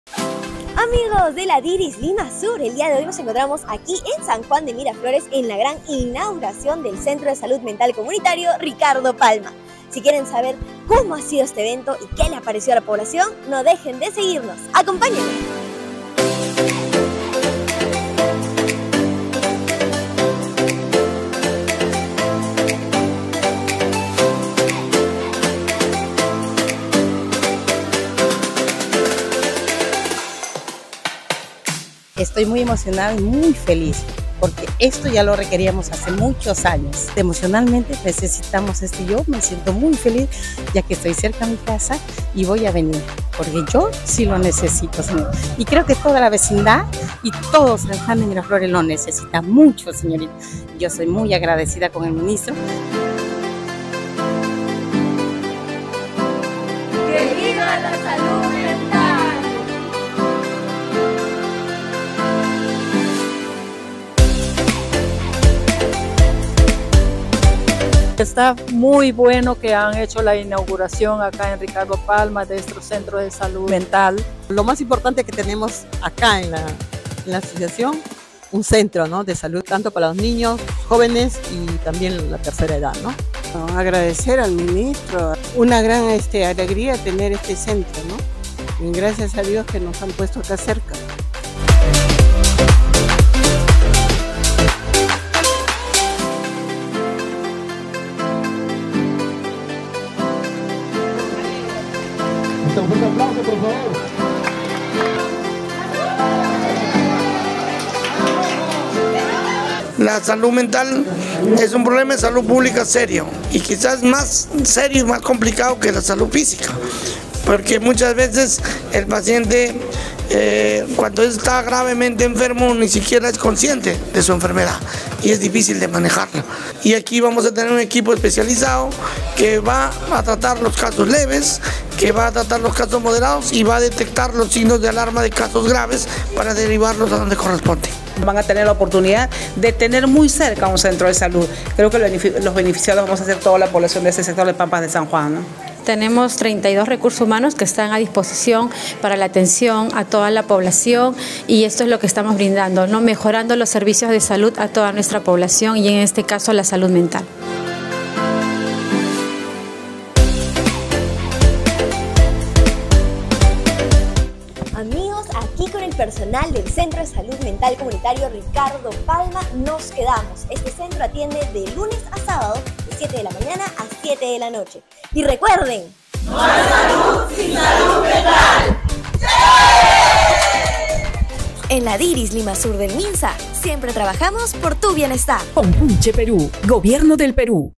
🎙️ ¡Diris Lima Sur en contacto con la gente!🎙️- Inauguración del CSMC Ricardo Palma
🎙️ ¡Diris Lima Sur en contacto con la gente!🎙️
Si te perdiste algún detalle de la inauguración del Centro de Salud Mental Comunitario Ricardo Palma en SanJuanDeMiraflores, ¡aquí te lo contamos!